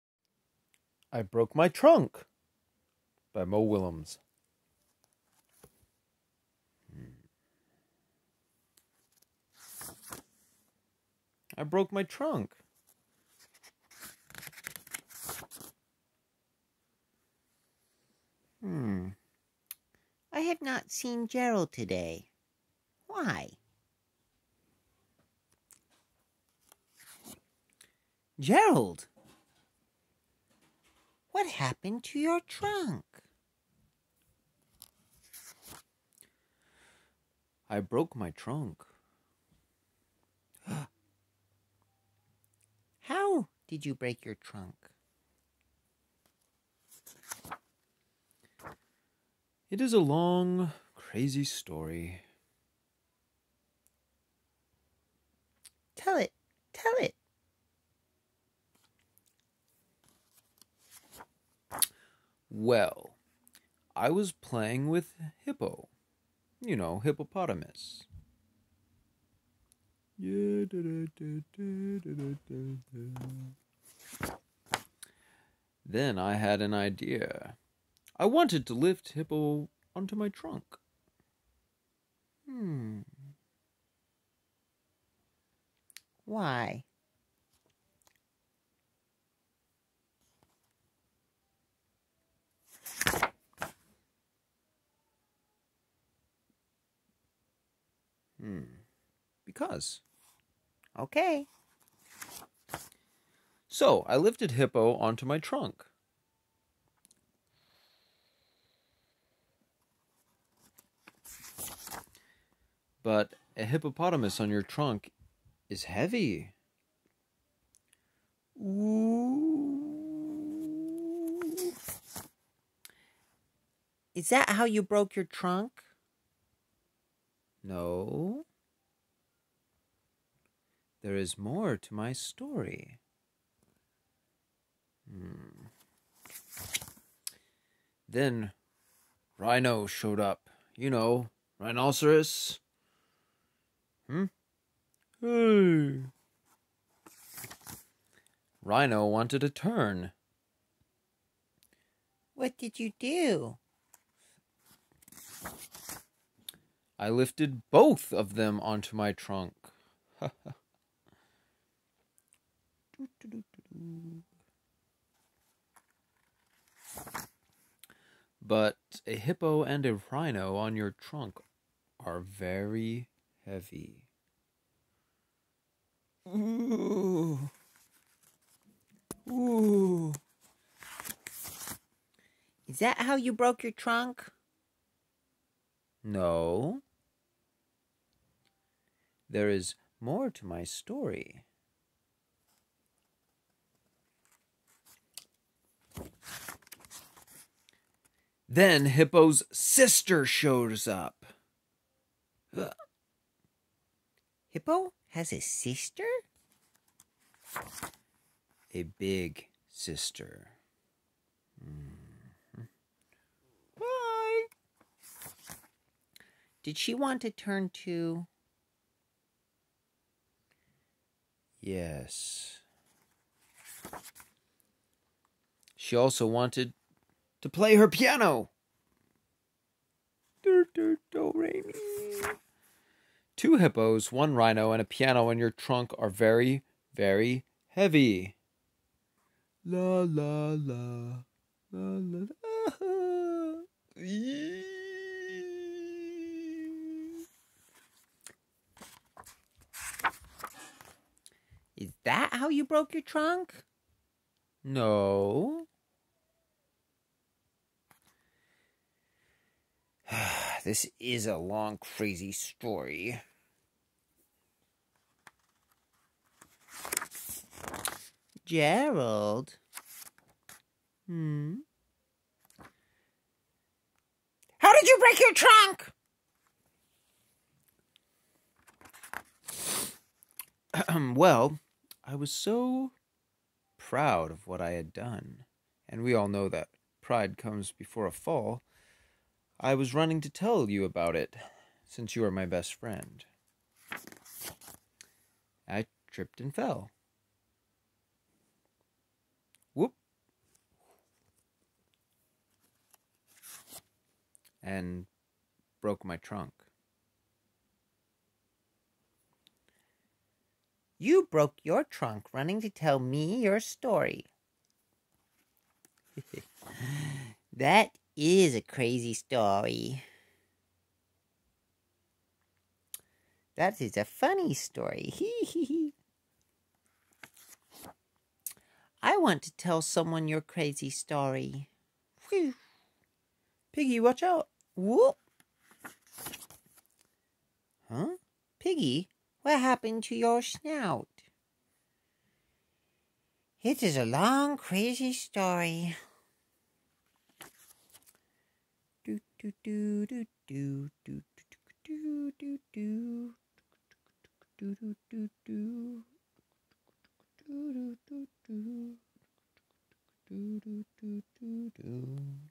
I Broke My Trunk!
by Mo Willems
Read by Sean Gray